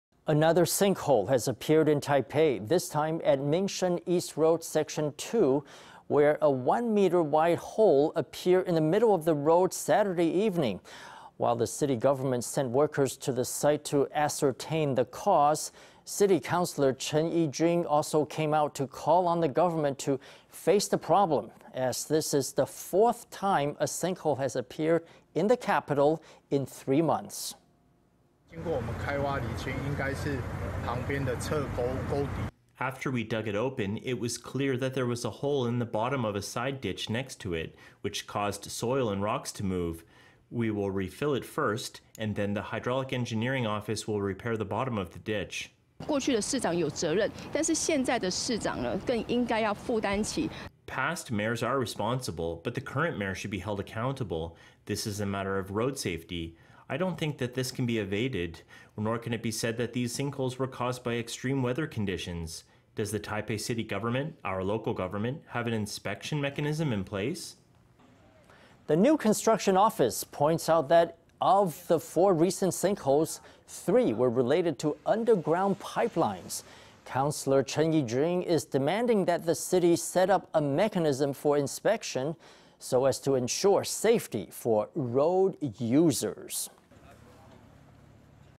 Sinkhole at Minsheng E. Rd., the fourth to appear in Taipei in three months
Another sinkhole has appeared in Taipei. This time at Minsheng East Rd. Section 2 where a one-meter-wide hole appeared in the middle of the road Saturday evening. While the city government sent workers to the site to ascertain the cause, city councilor Chen E-jun also came out to call on the government to face the problem, as this is the fourth time a sinkhole has appeared in the capital in three months.

Lin Kun-hu
New Construction Office, Taipei City Public Works Dept.
After we dug it open, it was clear that there was a hole in the bottom of a side ditch next to it, which caused soil and rocks to move. We will refill it first, and then the Hydraulic Engineering Office will repair the bottom of the ditch.

Chen E-jun
Taipei City councilor (DPP)
Past mayors are responsible, but the current mayor should be held accountable. This is a matter of road safety. I don’t think this can be evaded, nor can it be said that these sinkholes were caused by extreme weather conditions. Does the Taipei City Government -- our local government -- have an inspection mechanism in place?

The New Construction Office points out that of the four recent sinkholes, three were related to underground pipelines. Councilor Chen E-jun is demanding that the city set up a mechanism for inspection, so as to ensure safety for road users.